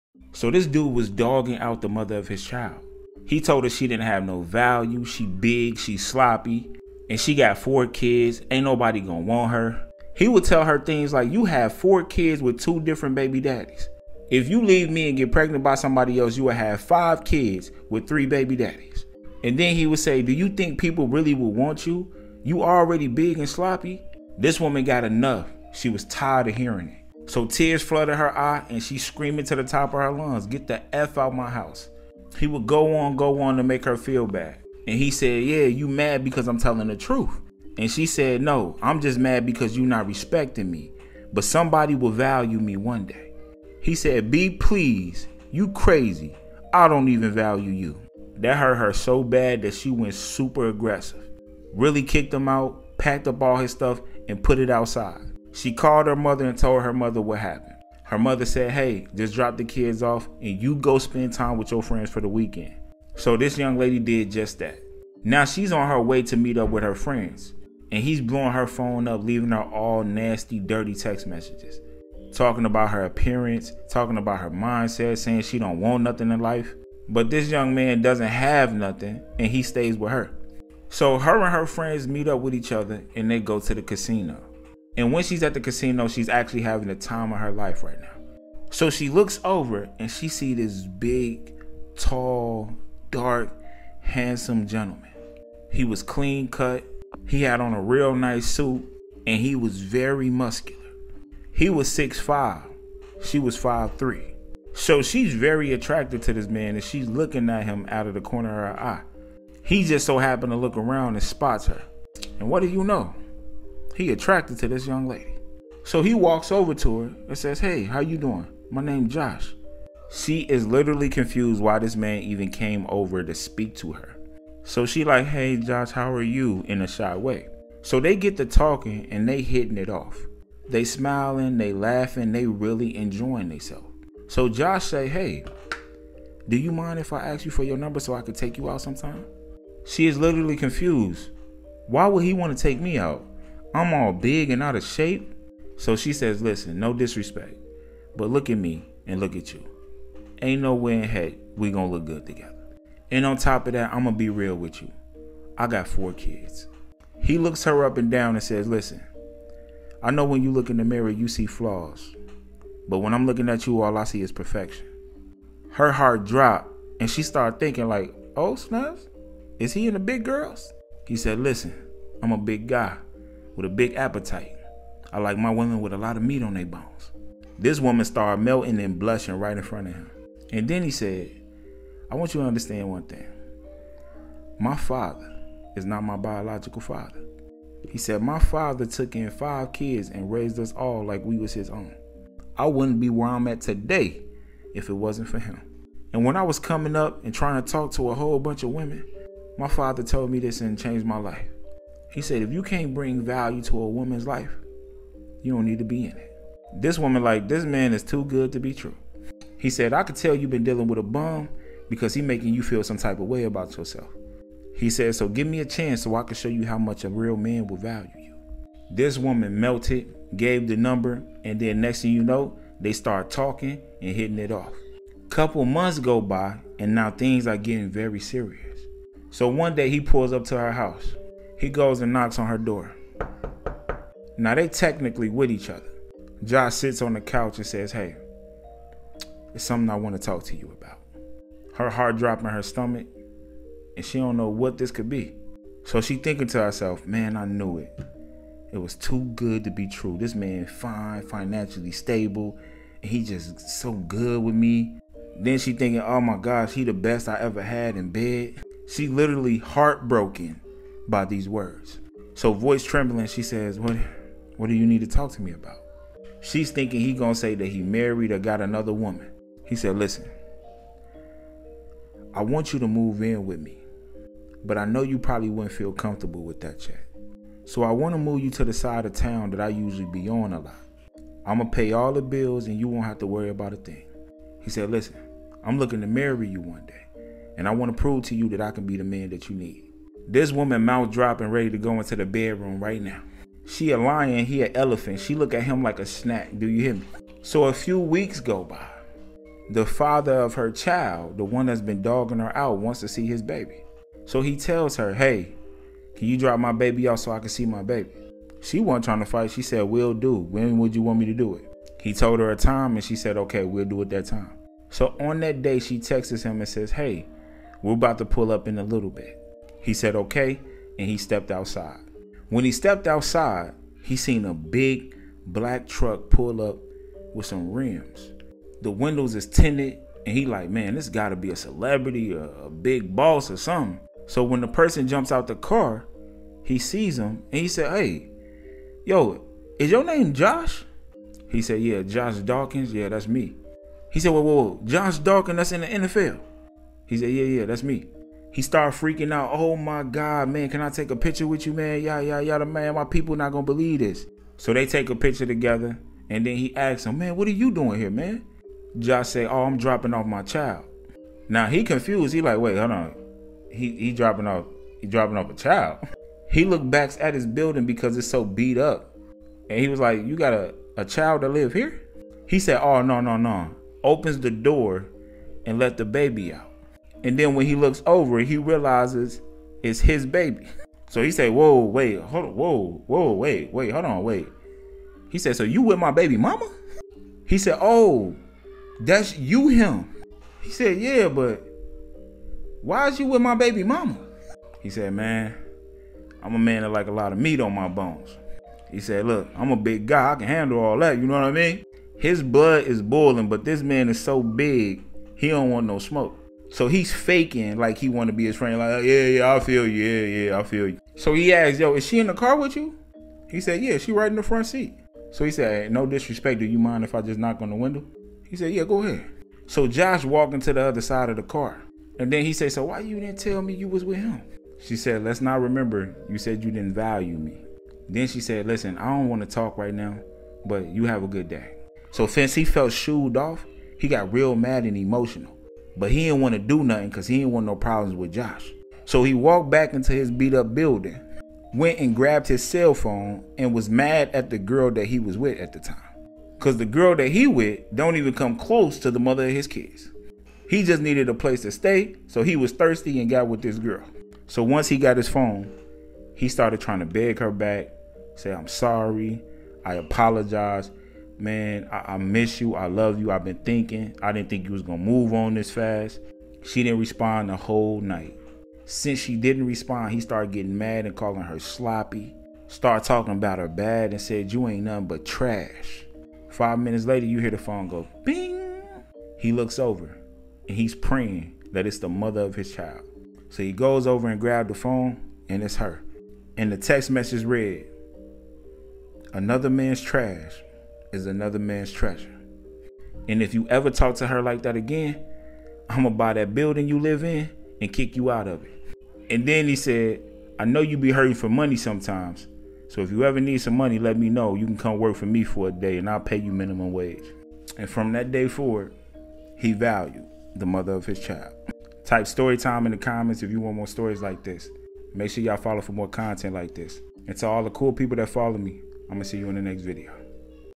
If you don't appreciate the mother of your child, someone else will come along who does
If you don't appreciate the mother of your child, someone else will come along who does. Treating her poorly not only affects her but also the well-being of your child. Value and respect her, or risk losing her to someone who sees her worth and treats her with the love and care she deserves. It's a harsh lesson in realizing too late what you had